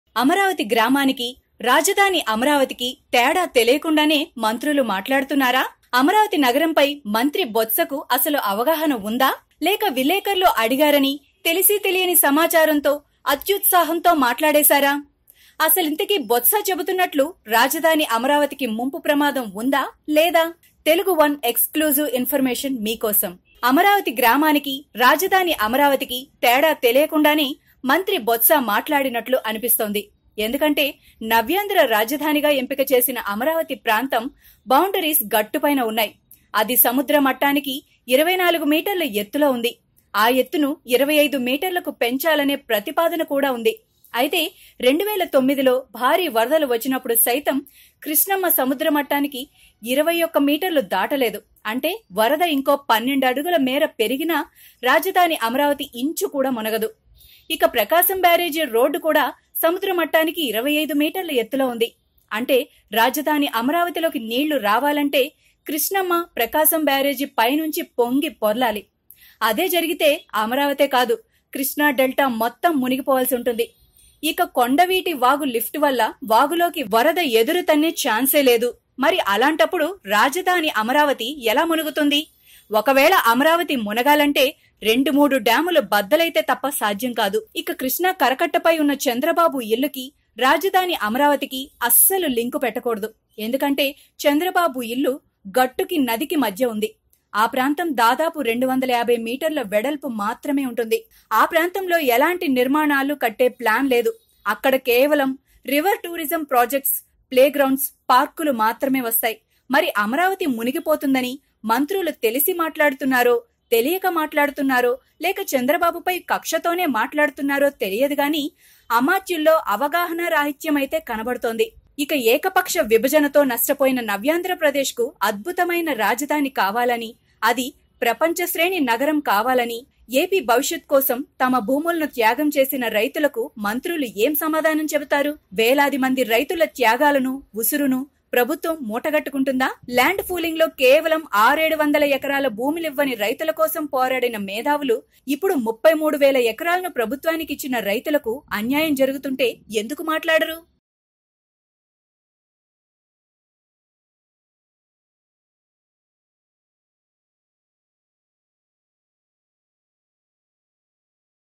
అమరావతి మునిగే ఛాన్సే లేదు…| No Chances of Amaravati Drowning |TeluguOne Exclusive Report| NewsOne
Seems like the ministers of YSRCP party, especially Botsa Satyanarayana just spoke out of enthusiasm & not with evidence or research over the Amaravati issue. While the YSRCP party is busy promoting that AP capital Amaravati is on the verge of drowning, TeluguOne exclusive survey says that it cannot happen so easily! To know our analysis, watch the video.